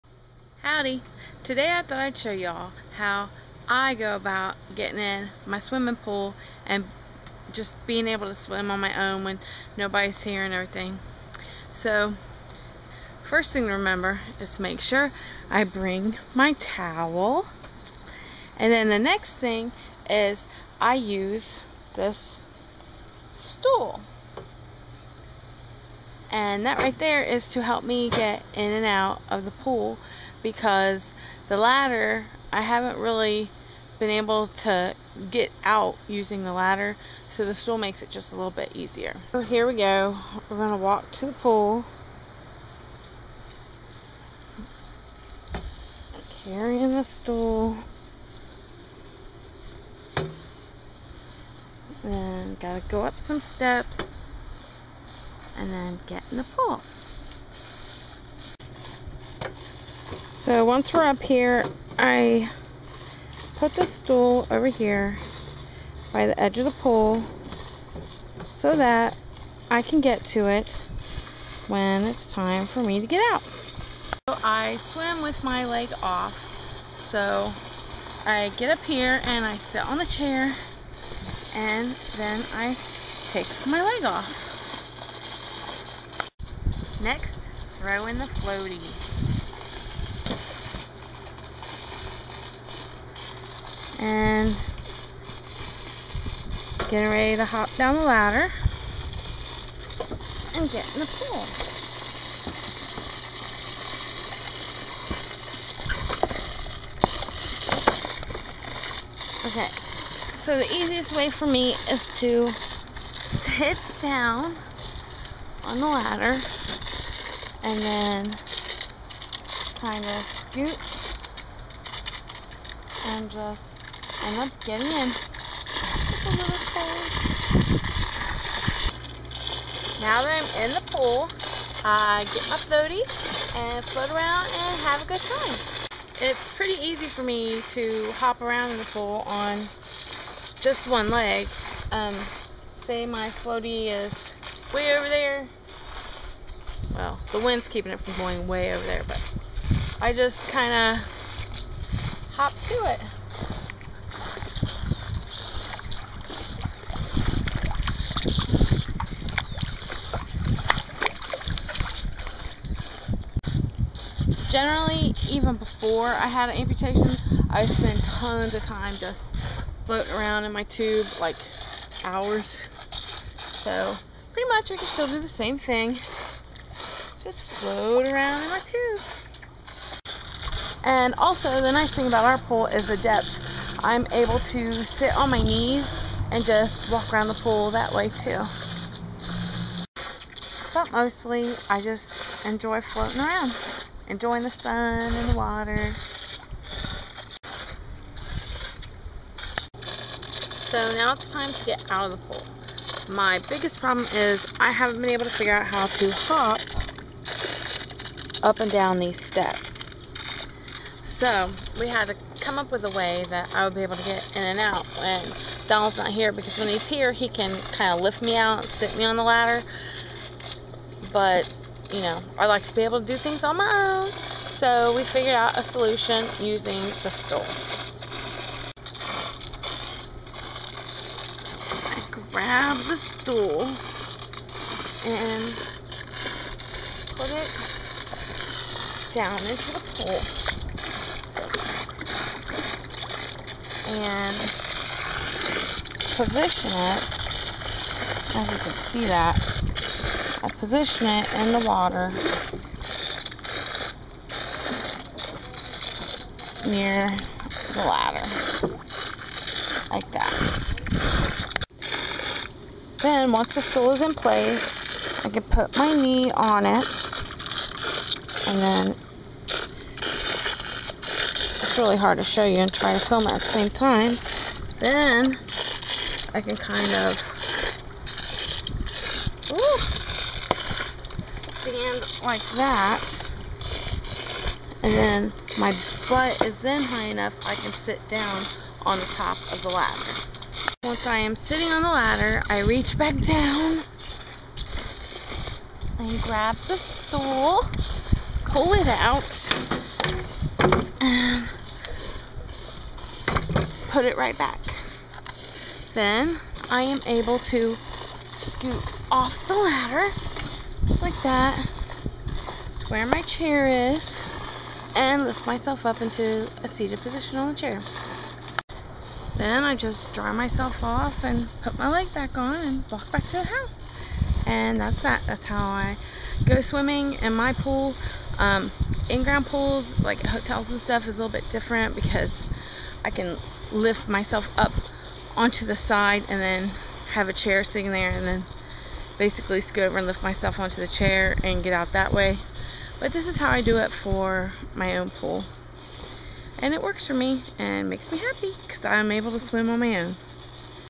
Navigating my pool as a LBK Amputee
I'm a recent LBKA figuring out how to do the things I love as close to the same way as I use to do them. It's not that I can't do things now, it's just about figuring out how to do things a different way.